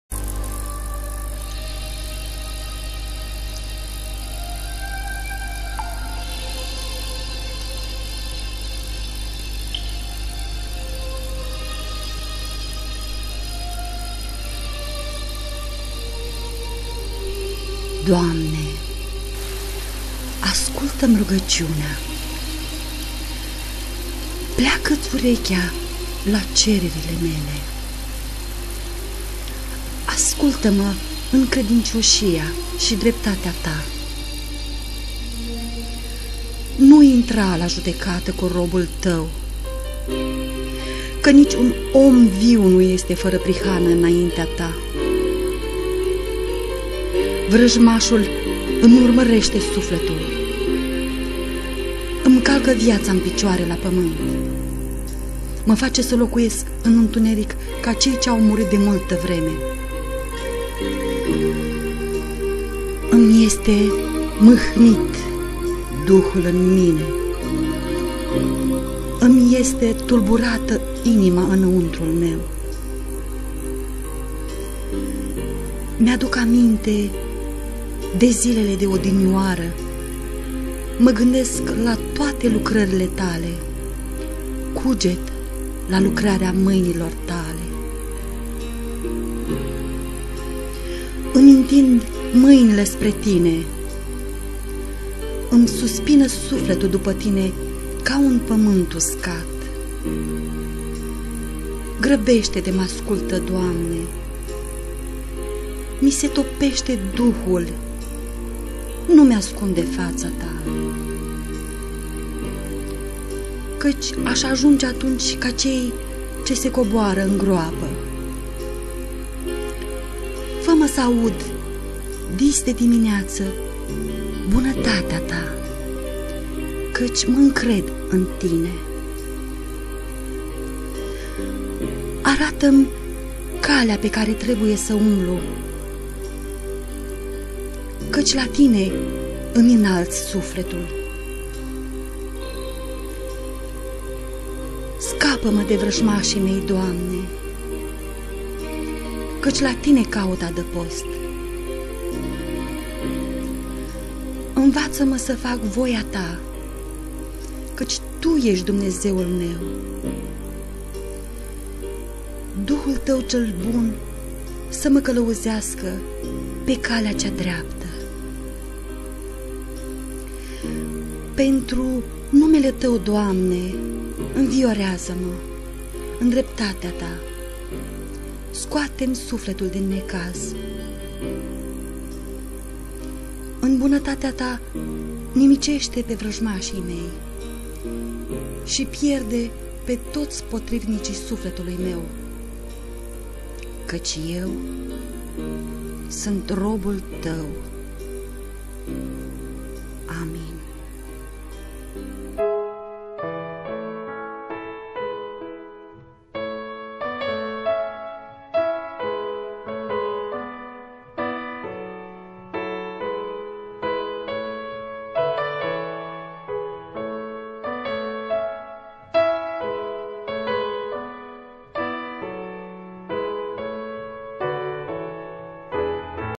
Pentru Numele Tau, Doamne, invioreaza-ma!- For your name's sake, O LORD, preserve my life!
Fa-ma sa aud dis-de-dimineata bunatatea Ta, caci ma incred in Tine. Arata-mi calea pe care trebuie sa umblu, caci la Tine imi inalt sufletul.
 Scapa-ma de vrajmasii mei, Doamne, caci la Tine caut adapost.
 Invata-ma sa fac voia Ta, caci Tu esti Dumnezeul meu. Duhul Tau cel bun sa ma calauzeasca pe calea cea dreapta!
. Pentru Numele Tau, Doamne, invioreaza-ma; in dreptatea Ta, scoate-mi sufletul din necaz! PSALMUL 143